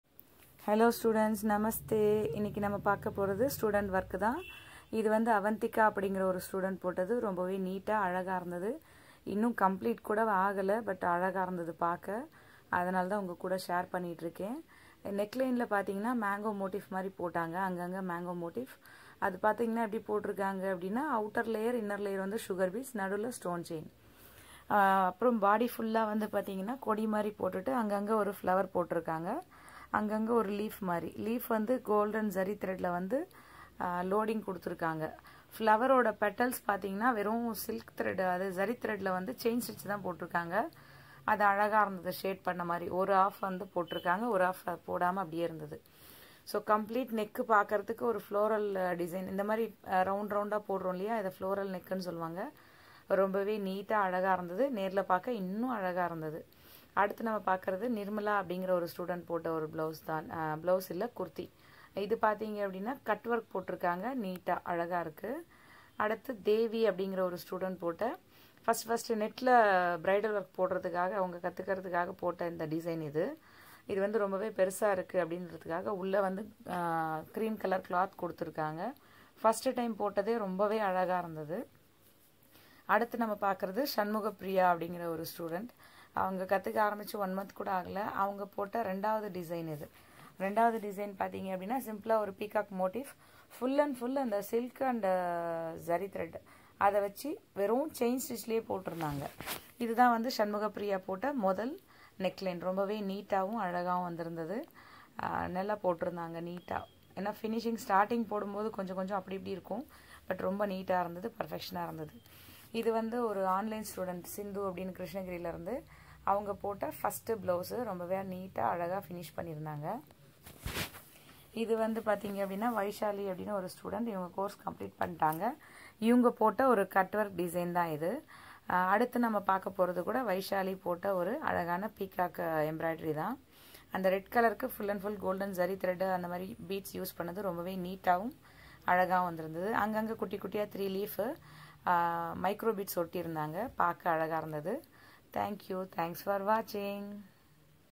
simpleaariwork | Students creativity | மாணவர் கைவண்ணம் | neck designs
Simple Aari embroidery neck designs by students with short time.